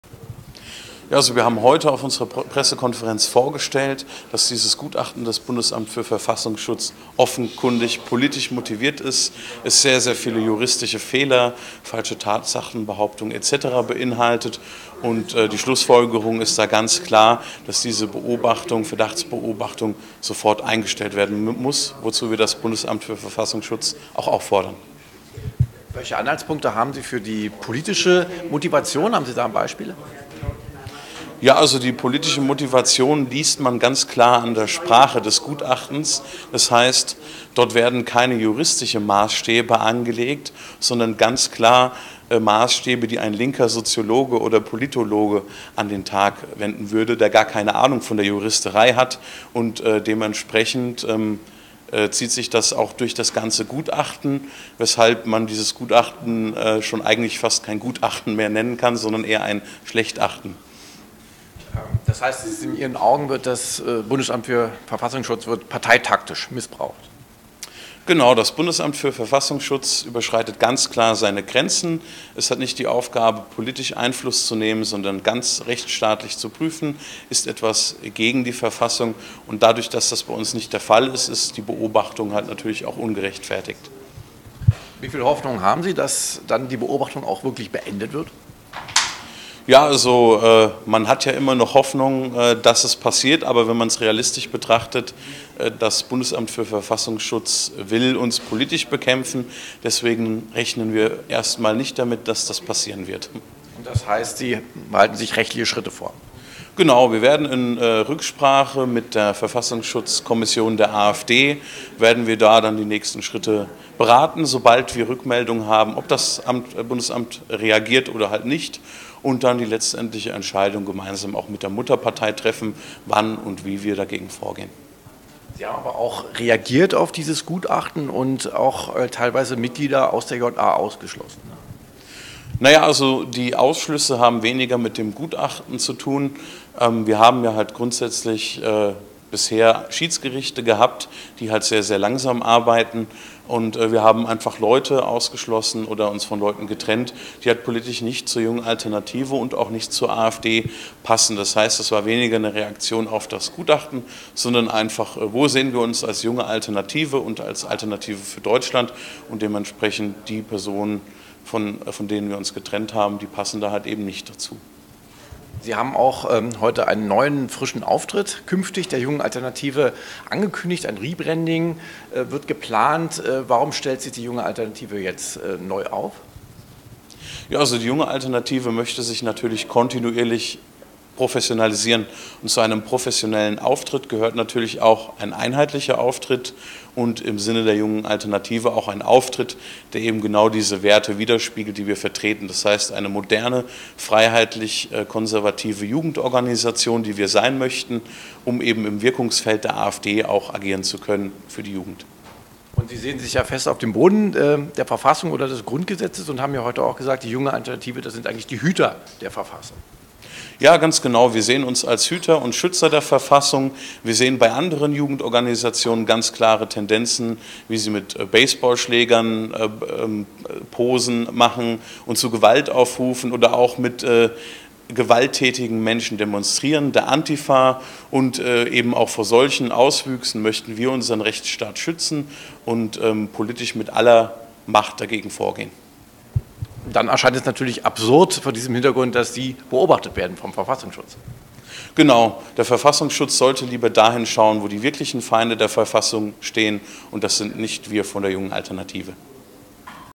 ❝BfV-Gutachten ist politisch motiviert!❞ | Damian Lohr
Im Anschluss an die Pressekonferenz der Jungen Alternative am 25. Juni 2019 in Berlin fasste der JA-Bundesvorsitzende Damian Lohr gegenüber AfD Kompakt TV noch einmal zusammen, warum er die Beobachtung durch das Bundesamt für Verfassungsschutz BfV für parteitaktisch motiviert hält und wie er sich gegen eine weitere Beobachtung wehren will. Außerdem kündigte er einen Relaunch der Marke JA an.